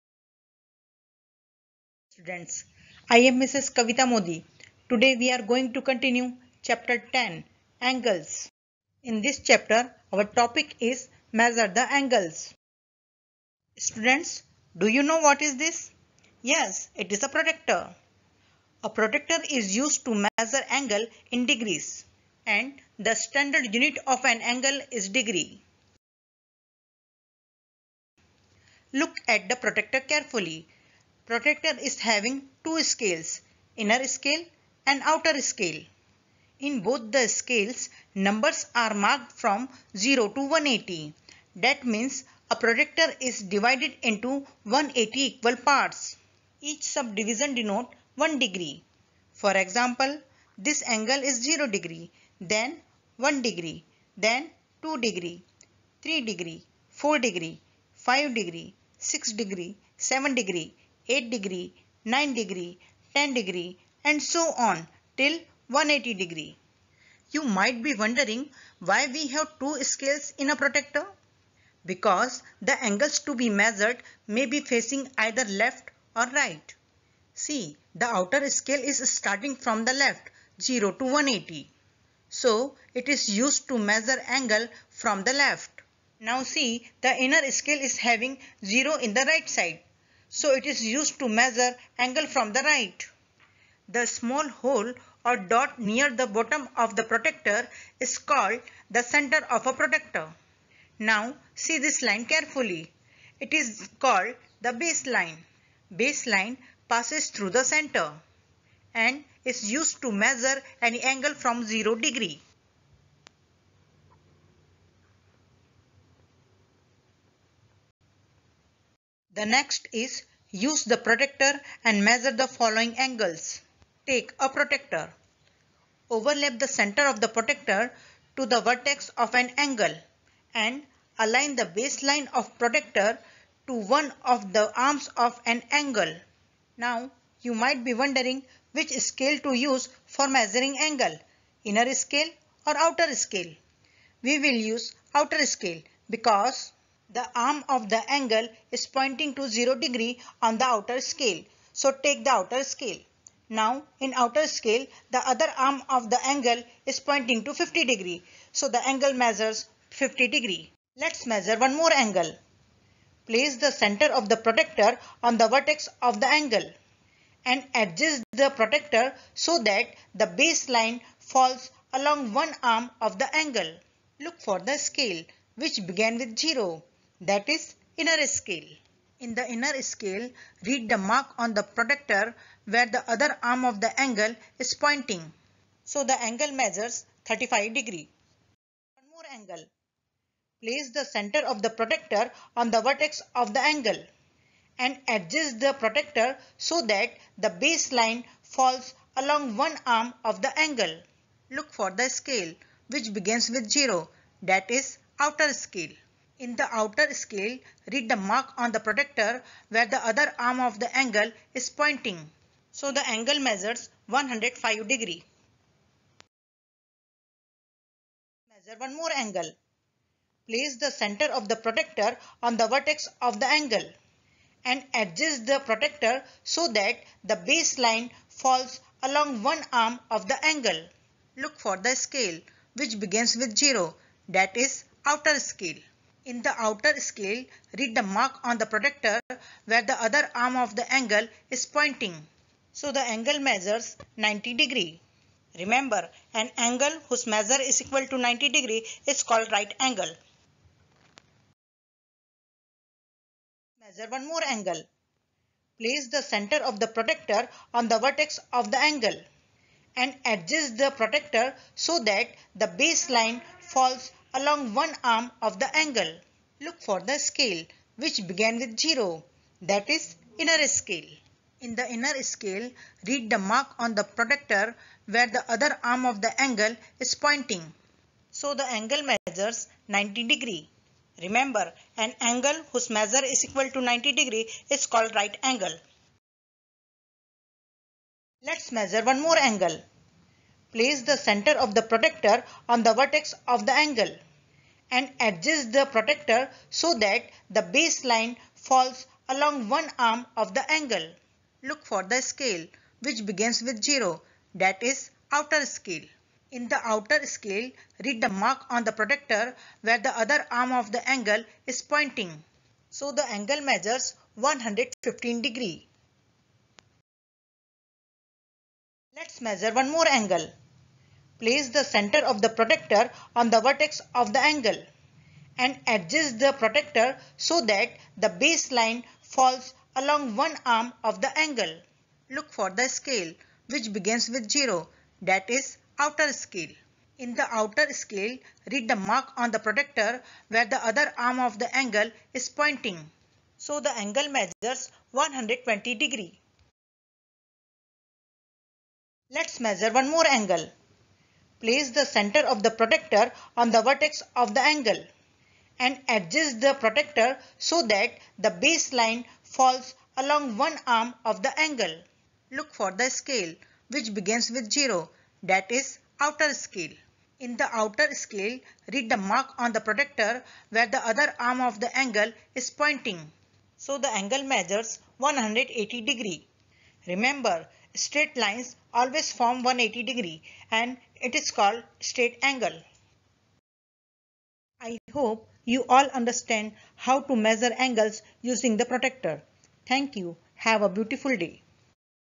Ls-10, Angles(Part-2) by Mrs. Kavita Modi.
Class: 4
Subject: Mathematics
Chapter: 10, Angles(Part-2)
Topic: WK-3, Q.1, Measuring angles.
Teacher: Mrs. Kavita Modi